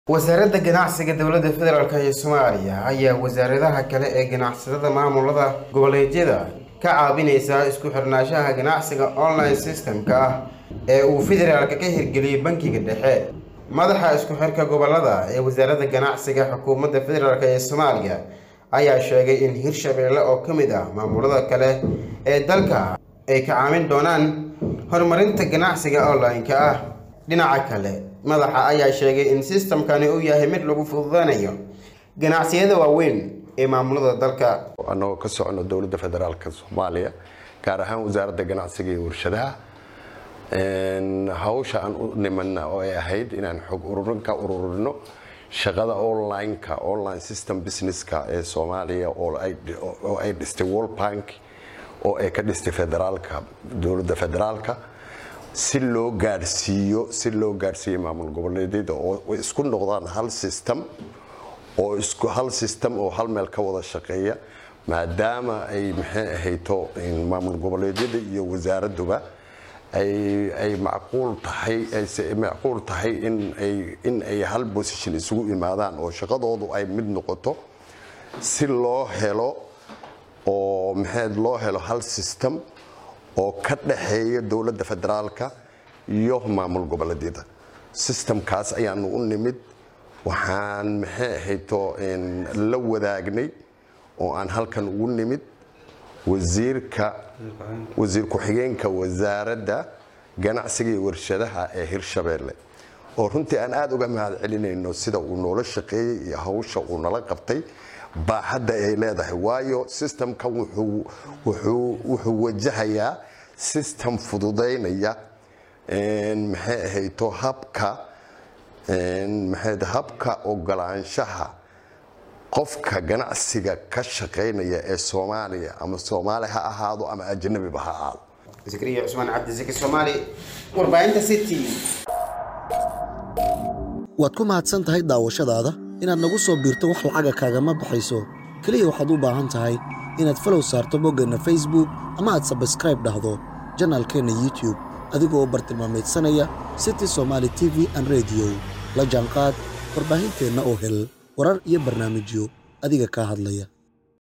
Xukuumada Soomaaliya oo Wasaaradaha Ganacsiga Dalka ka Caabonaysa Ganacsiga Online Systemka ah Zeki
Xukuumada Soomaaliya oo Wasaaradaha Ganacsiga Dalka ka Caabonaysa Ganacsiga Online Systemka ah Zeki Somali ayaa Kawaramaya